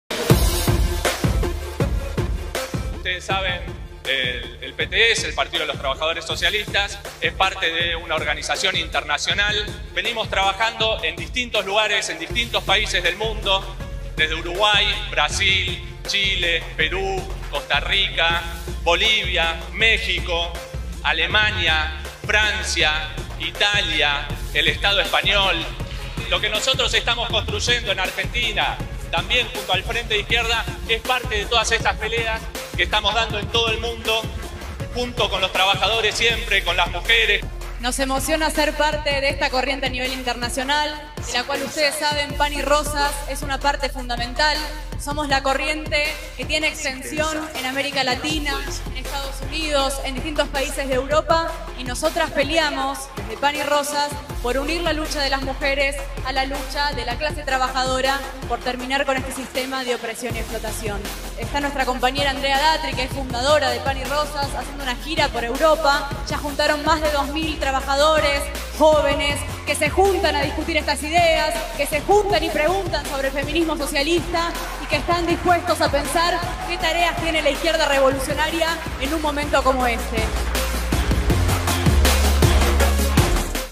Wahlkampagne der PTS: "Unser Kampf ist nicht nur in Argentinien, sondern international"
Beim Wahlkampfauftakt der Partei Sozialistischer Arbeiter*innen (PTS) in Argentinien am vergangenen Wochenende spielte die Idee des Internationalismus eine zentrale Rolle.